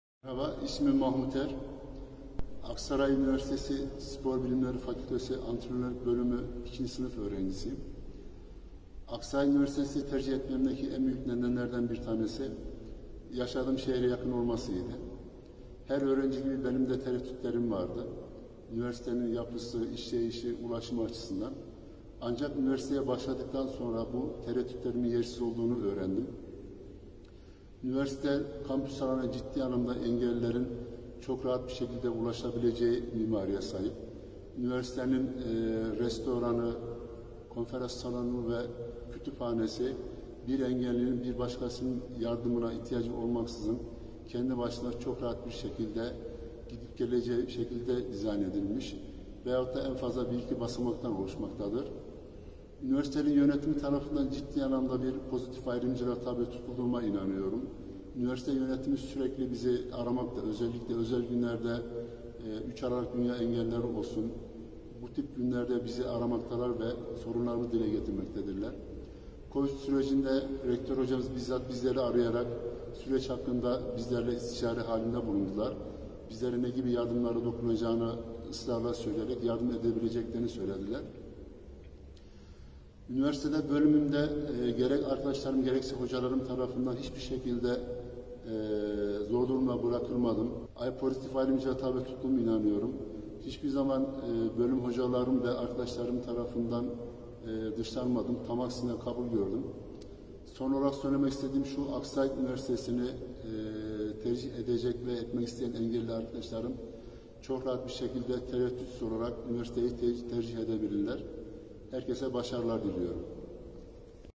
Engelsiz Üniversite
Engelleri kaldıran ASÜ, iki yılda dört bayrak ödülü aldı. ASÜ, 2018 yılında, kampüs içinde yapılan düzenlemelerin ardından, mekânda erişim anlamına gelen turuncu bayrak ödülü aldı. 2020 yılında ise Eğitim Fakültesi ve Fen Edebiyat Fakültesi turuncu bayrak ödülüne layık görülürken, Eğitim Fakültesi buna ek olarak eğitim-öğretim ortamlarına ve materyallerine erişim anlamına gelen yeşil bayrak ödülü aldı. ASÜ’de toplam 21 bina, turuncu bayrak ödülü için adaylık kazandı. Yükseköğretim Kurulu (YÖK) tarafından verilen bayrak ödülleri, engelli öğrencilerin binalara erişiminin yanı sıra eğitim-öğretim olanaklarına ve ders materyallerine erişimlerini de kapsıyor.